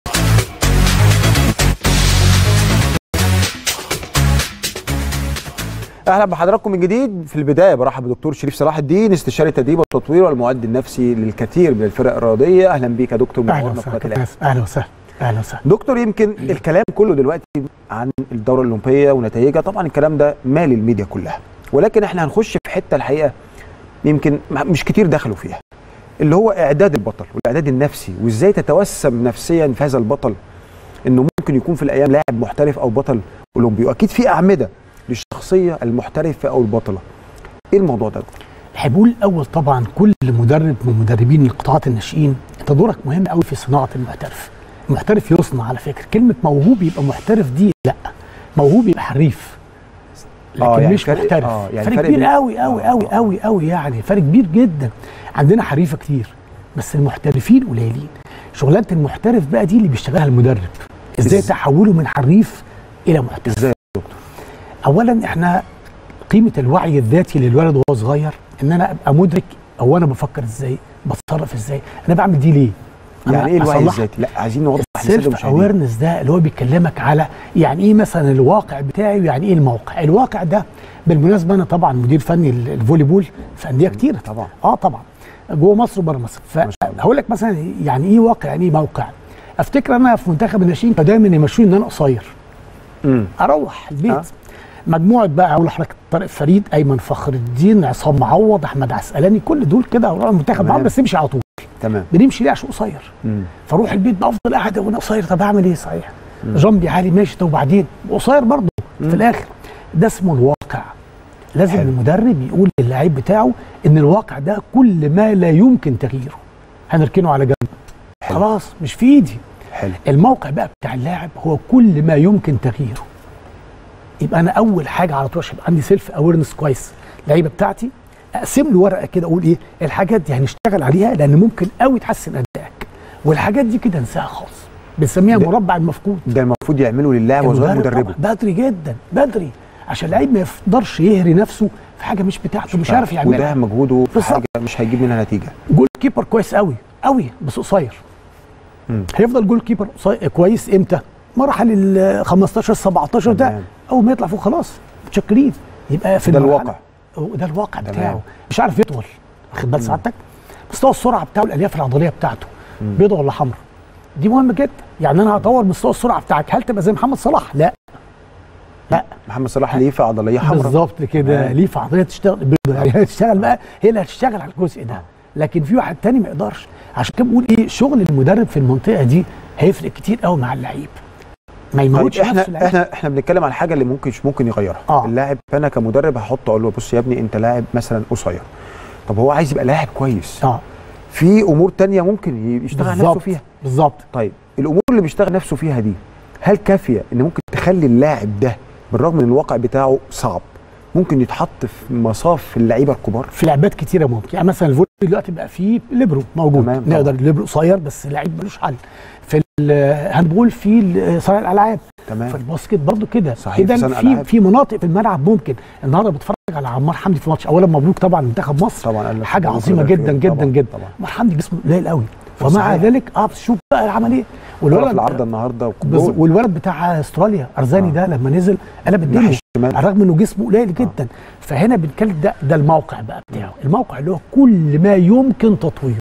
شريف صلاح الدين: نمتلك اللاعب الحريف.. ولكن الكمال في الشخصية الاحترافية
شريف صلاح الدين: نمتلك اللاعب الحريف.. ولكن الكمال في الشخصية الاحترافية  | الأبطال